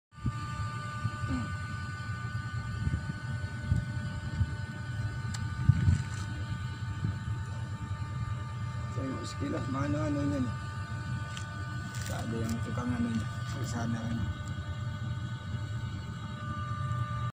MT. BITUMEN HIKARI VOY 16/19.(2)
Proses Bongkar Aspal curah di Pelabuhan Muara Sabak.
Kapal sandar pada tanggal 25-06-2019 pukul 23:40 wib.
Dan mulai kegiatan bongkar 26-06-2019 jam 01:25.
Finish bongkar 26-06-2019 jam 14:40 dan berangkat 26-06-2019 jam 20:00 wib.